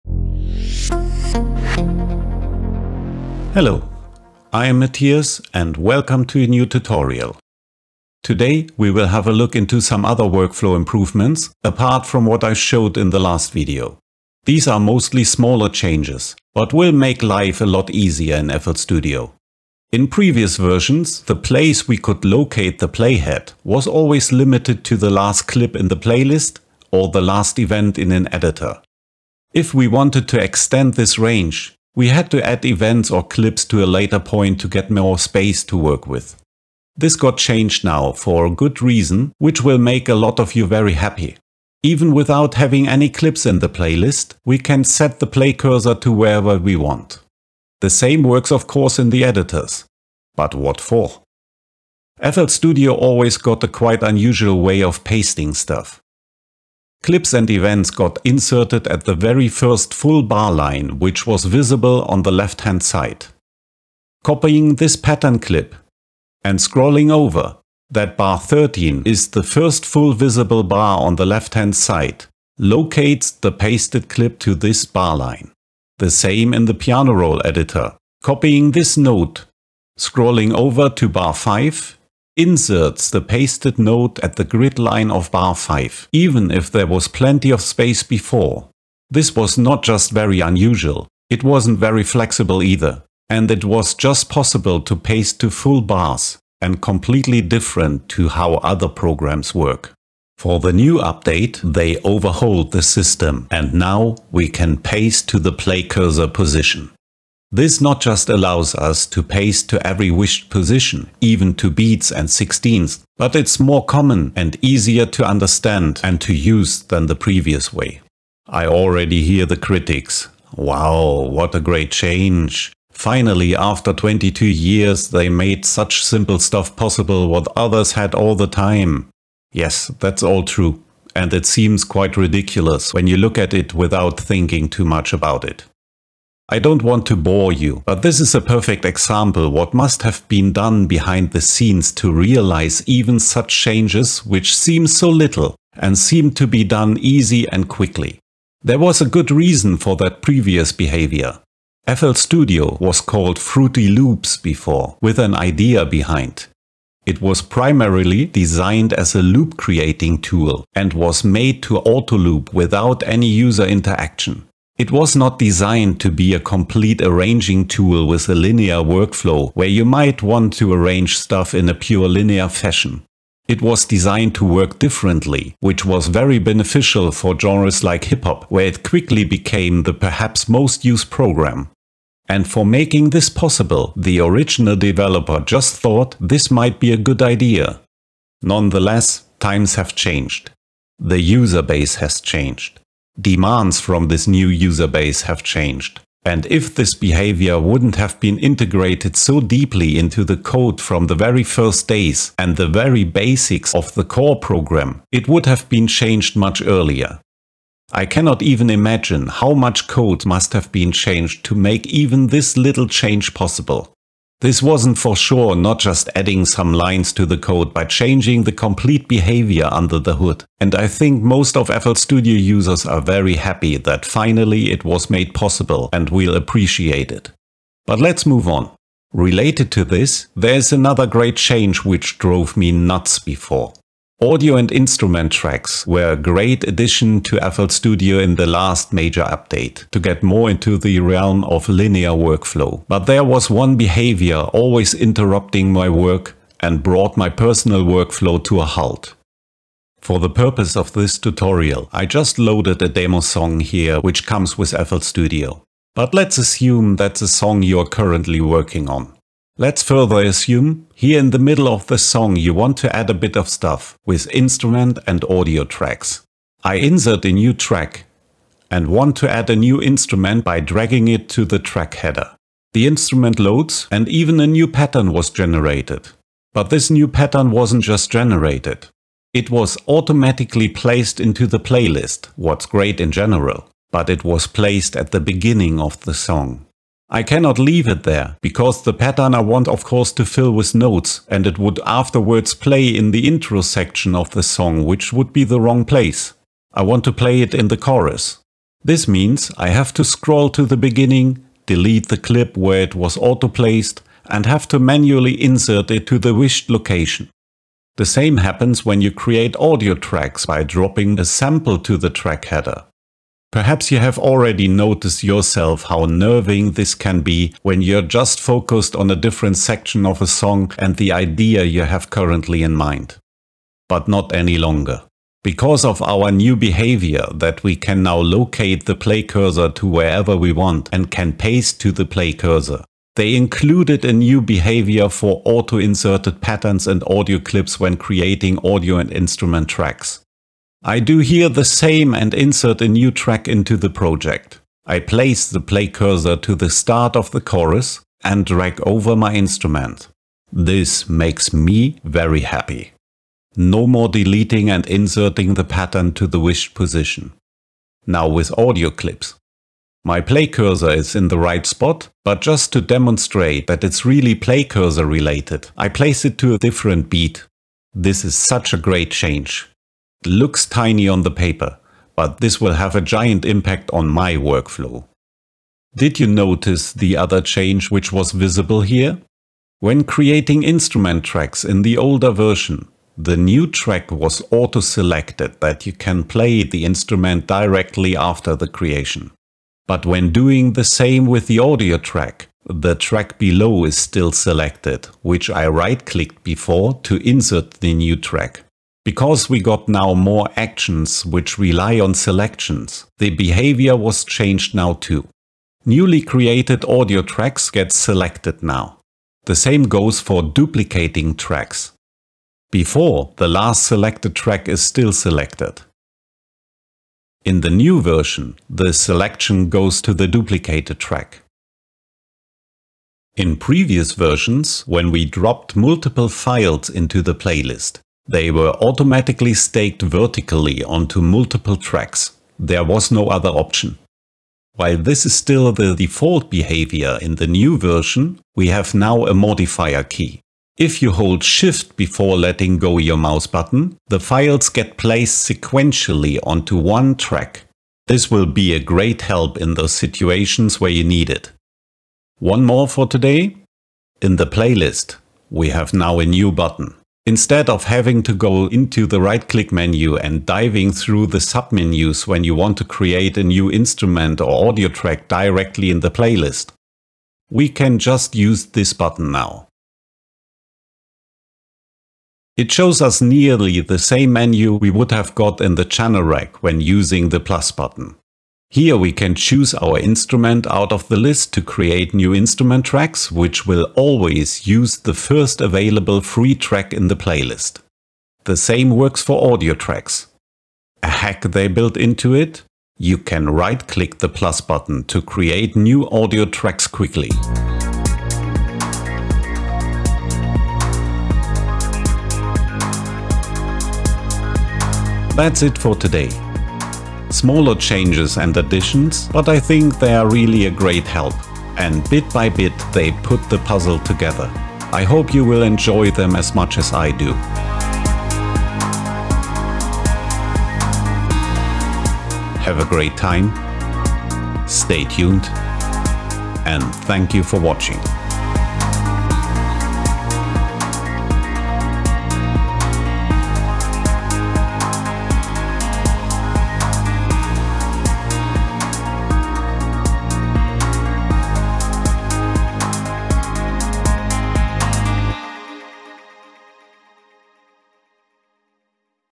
FL Studio 21 beta... What´s new! Great Workflow Improvements!
Let´s go over some smaller changes which make lie so much easier in the new version...enjoy!

0:00 Intro
0:19 Play cursor placement
0:51 New Copy/Paste
1:58 Some things to think about :-)
3:47 Autoplacement of clips
6:16 Audio and Cloned track selection
7:03 Sequential placement
7:32 "+"